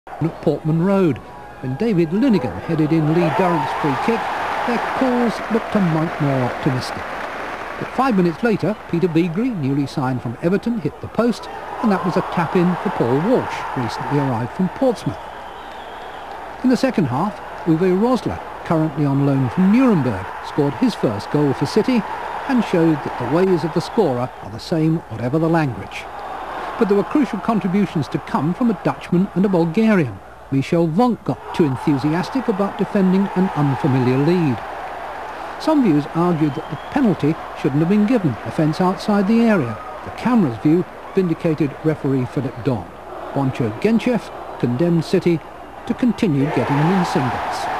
Ipswich Town 2 Manchester City 2, 29 March 1994
David Linighan's header gave Town the lead at Portman Road before goals from Paul Walsh and Uwe Rosler put the visitors in front. Bontcho Guentchev then converted a penalty to earn Ipswich a point.